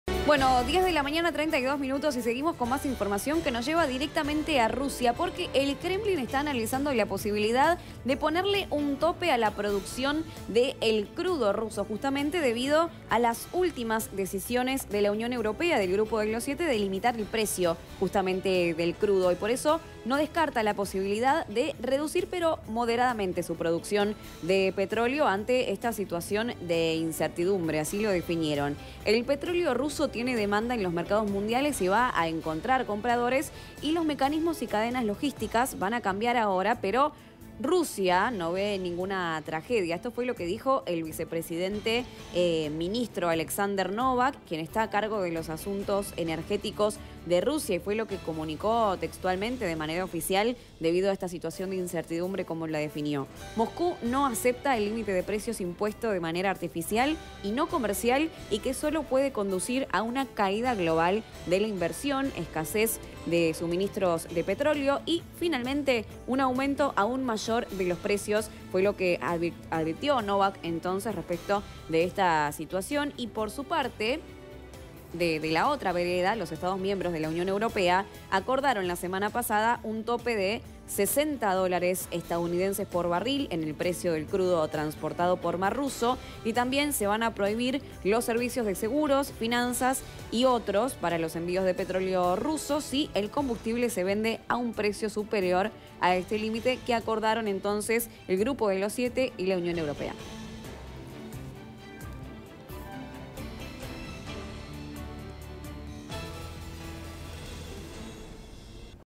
Rusia analiza hacer una reducción de la producción de petróleo
👉 Rusia estudia fijar un precio mínimo para sus ventas internacionales de petróleo en respuesta al tope establecido la semana pasada por los países del G7.